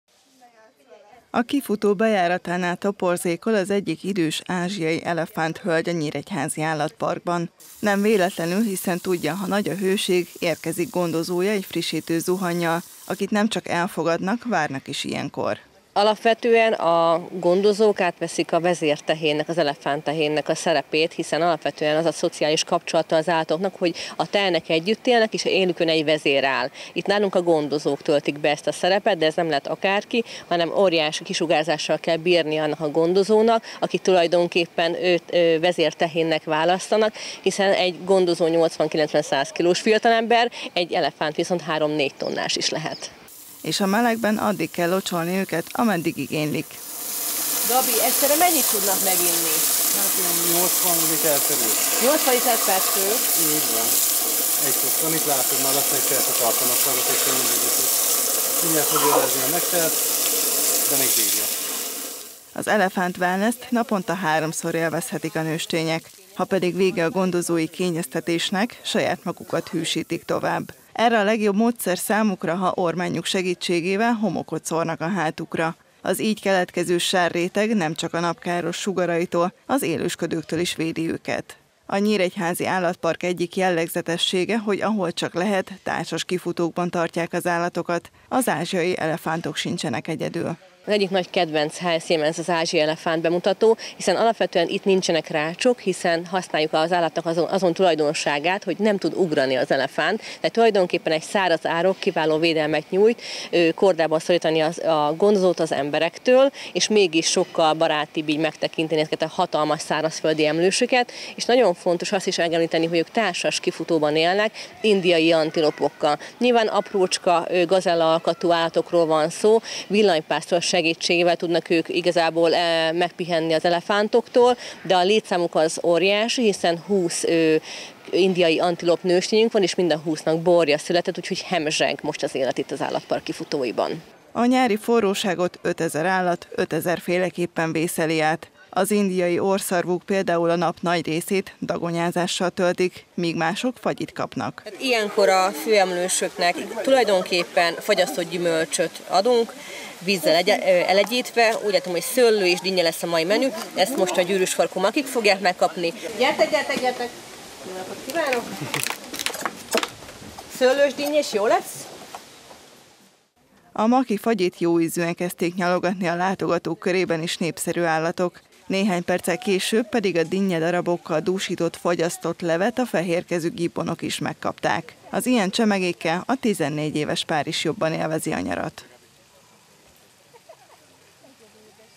Az állatok is hűsölnek
Talán kevesen tudják, de egyszerre akár 80 liter vizet is megiszik egy ázsiai elefánt a hőségben. A gyűrűsfarkú makik pedig fagyasztott szőlő és dinnyefagyit kapnak a kánikulában. A Nyíregyházi Állatpark gondozói nyáron is figyelnek az állatokra. Mint mondják, az ötezer állat, ötezer különböző igényt jelent.